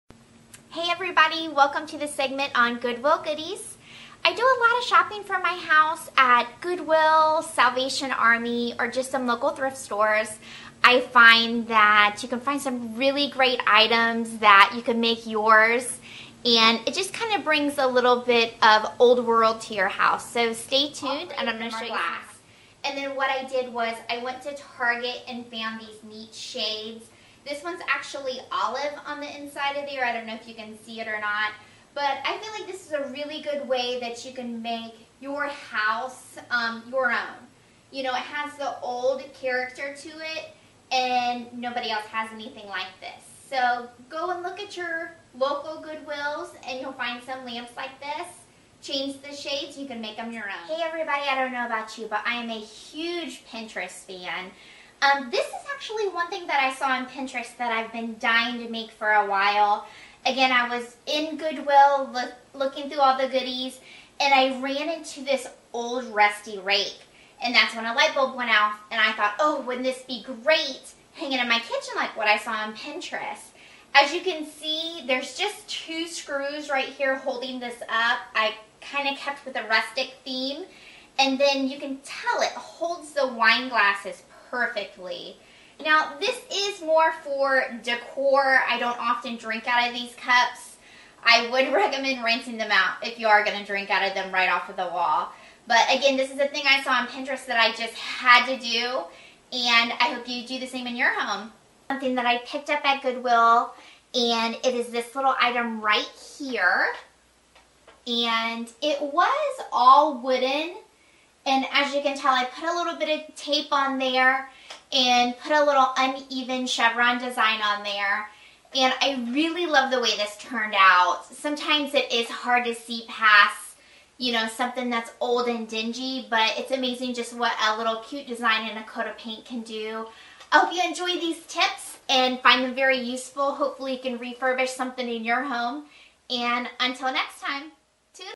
Decorating on a Budget- Goodwill Goodies...
Okay guys I finally figured out some editing software(sorry about the text that will pop up a few times- please ignore). I promise it will get better each time :) In this video you will find some of my Goodwill and Thrift Store goodies!! Hope you enjoy.

In this video you will see that I found just an old rake from Goodwill. For this project the older and the more aged the better. I was going for a "Rusty" Look!

(Lori Vandercrake)